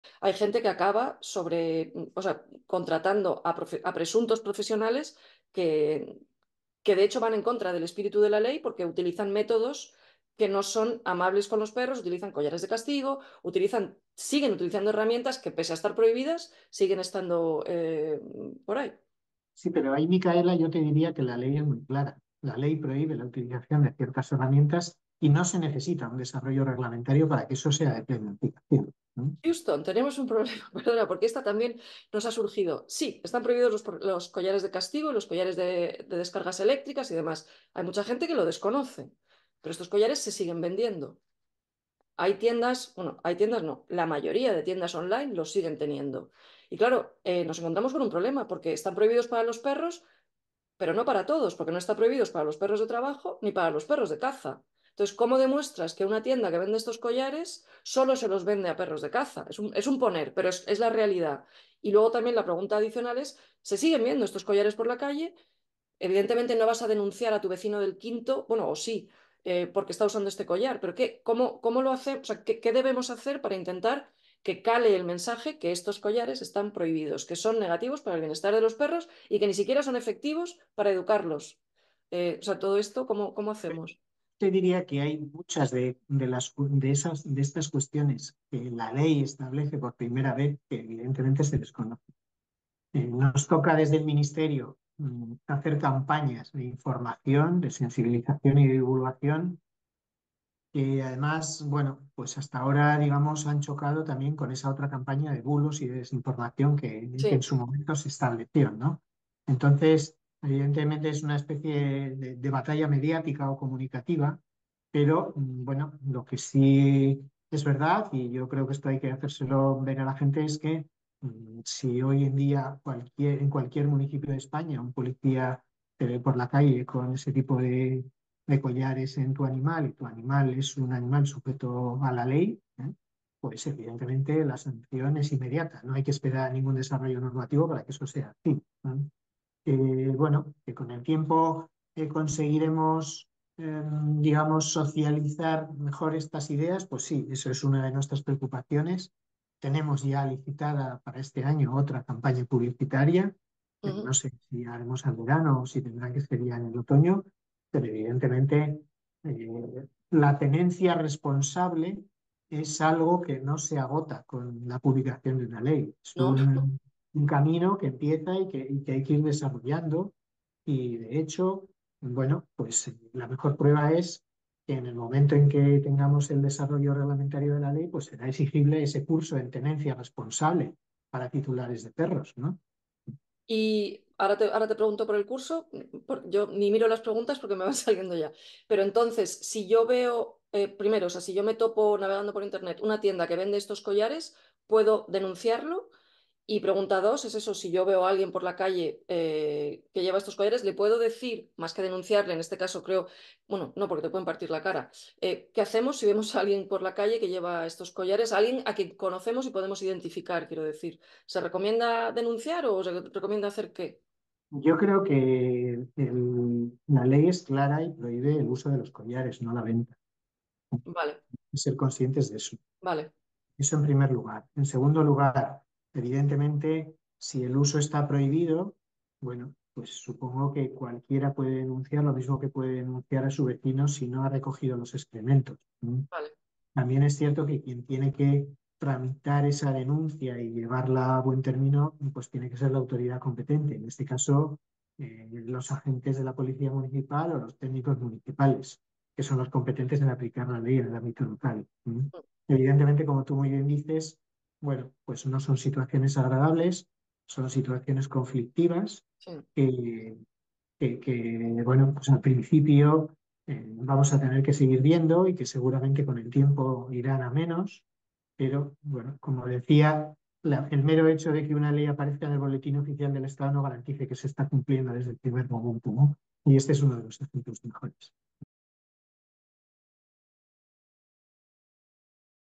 Está prohibido en España el uso de collares de castigo en perros, (pero no la venta)
Si hoy en día un policía en cualquier municipio de españa un policia te ve con un collar de ese tipo en tu animal, la sanción es inmediata, explica José Ramón Becerra, Director General de Derechos de los Animales.

La ley es muy clara y tajante al respecto, ha prohibido determinadas herramientas como los collares de castigo o de descargas y no se necesita desarrollo reglamentario.

Más información en SrPerro.com